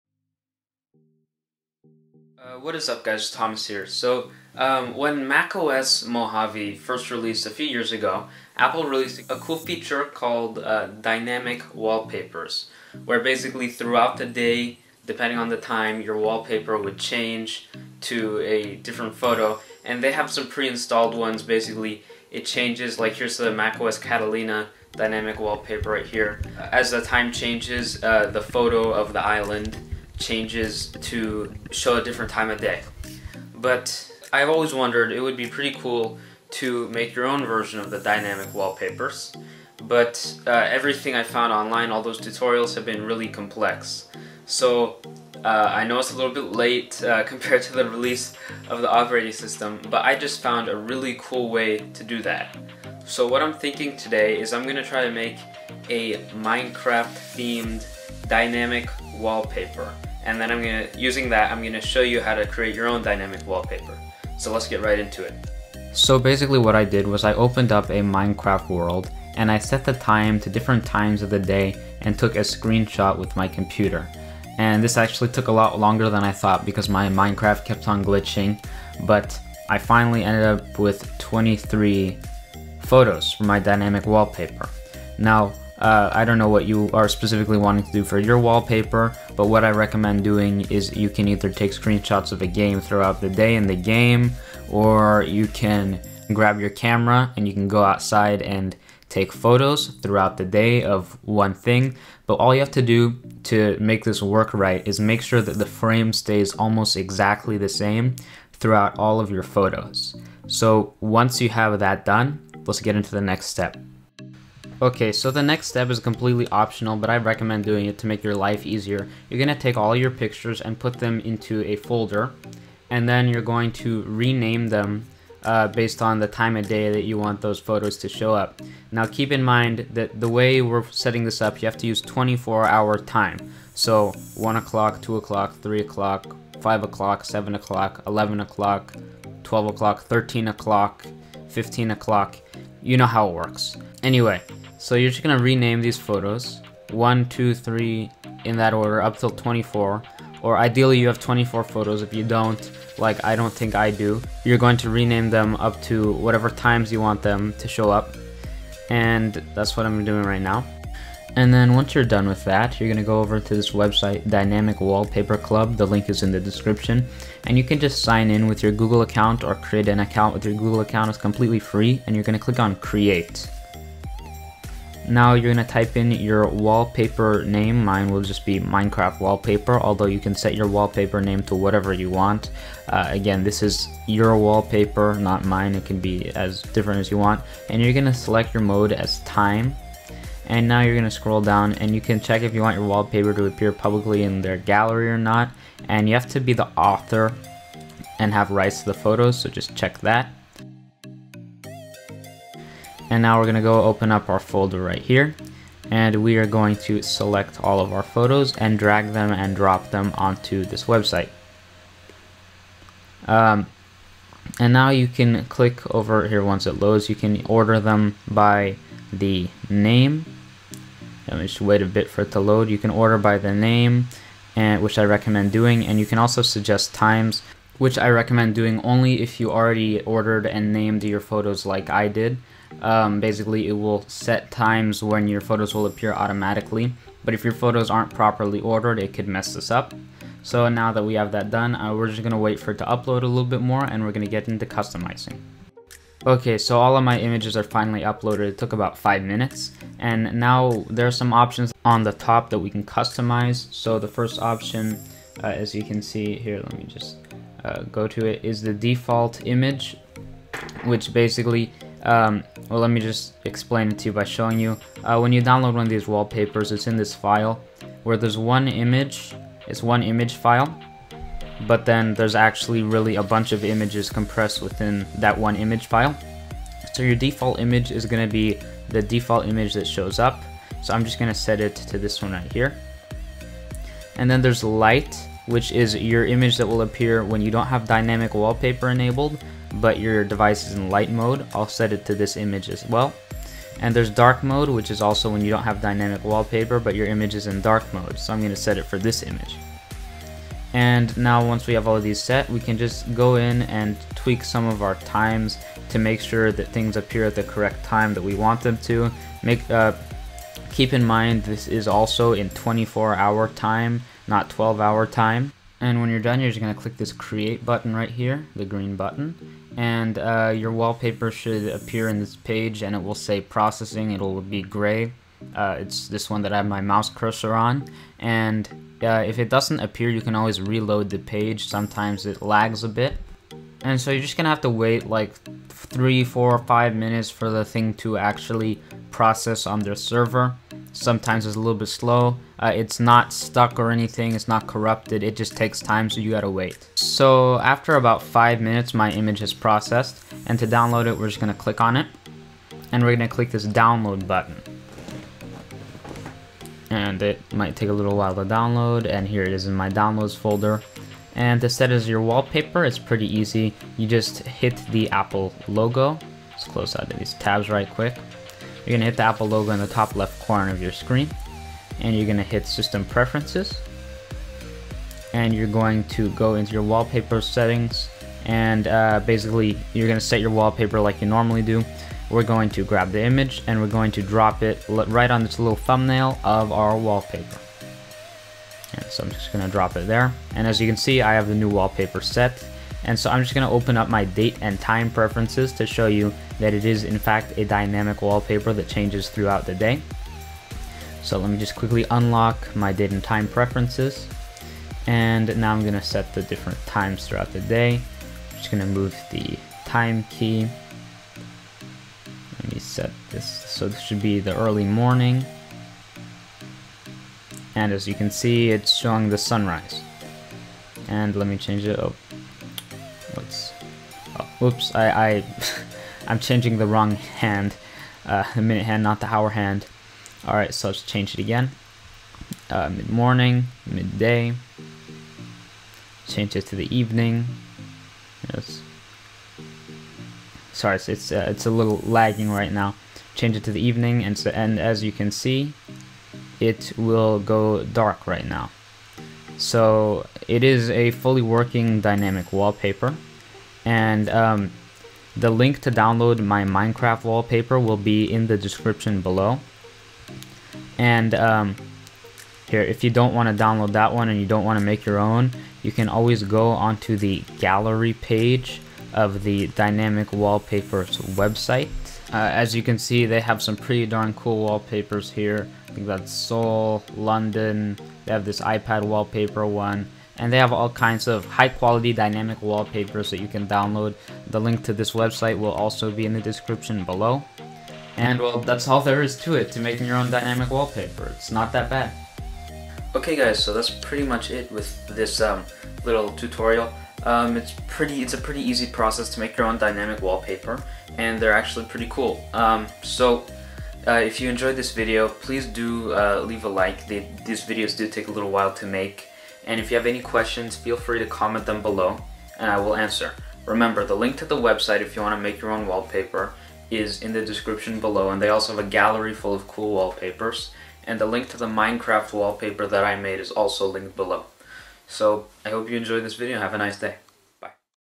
Easily Make Your Own Custom Dynamic Wallpapers on macOS Catalina
Hey everyone, today we will be going over how to easily create your own custom dynamic wallpapers for macOS Mojave or macOS Catalina. It's pretty easy to do, and I hope you enjoy the video!

If you need additional help, feel free to let me know.